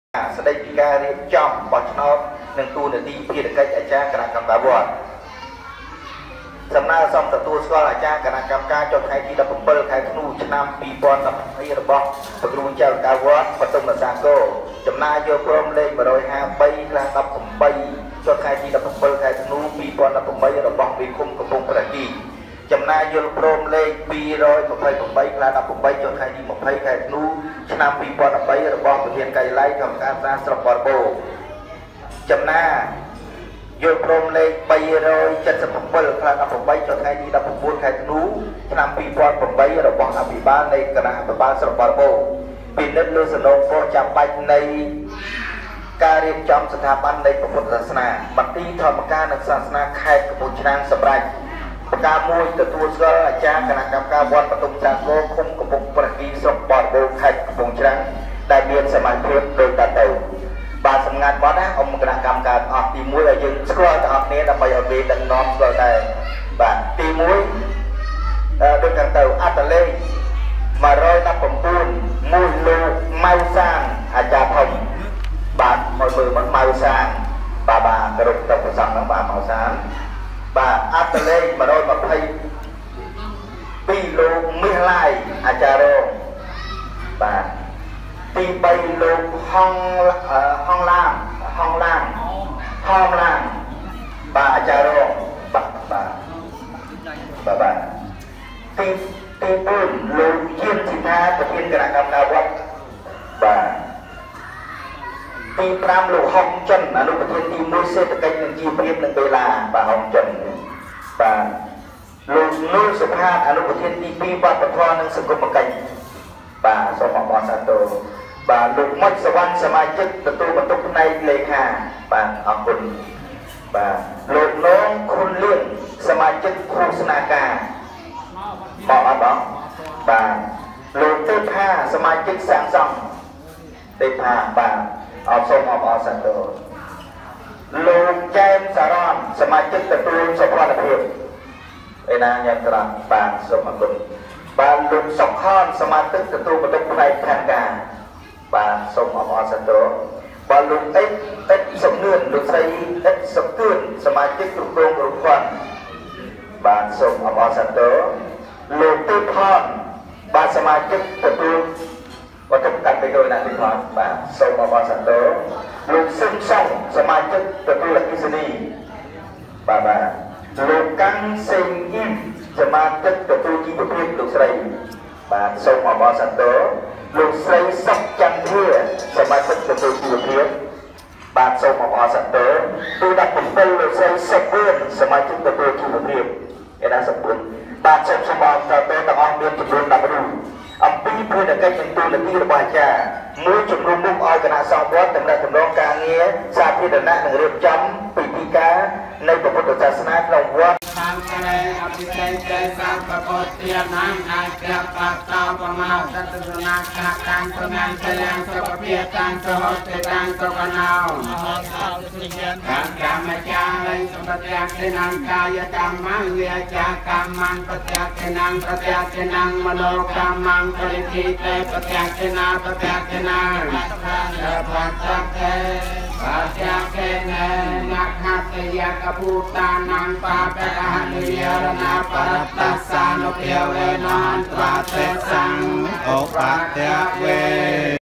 ឧទ្ទេសនាម និងប្រគល់ត្រាដល់ព្រះចៅអធិការ ទទួលស្គាល់អាចារ្យ គណកម្មការវត្តបទុម្ពសាគរ ស្រុកបរិបូណ៍ ឃុំ
ឧទ្ទេសនាម និងប្រគល់ត្រាដល់ព្រះចៅអធិការ ទទួលស្គាល់អាចារ្យ គណកម្មការវត្តបទុម្ពសាគរ ស្រុកបរិបូណ៍ ឃុំតាម៉ូវ